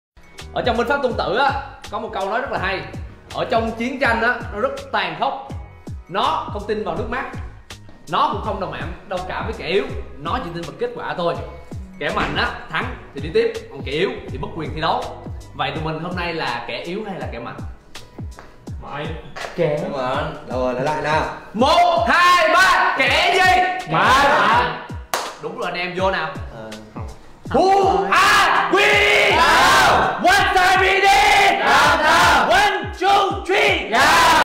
Cụ Tổ Warzone với Binh Pháp Tôn Tử đe dọa vị trí "HLV đạo lý" số 1 của Ngài Ren
Credit to: GAM Esports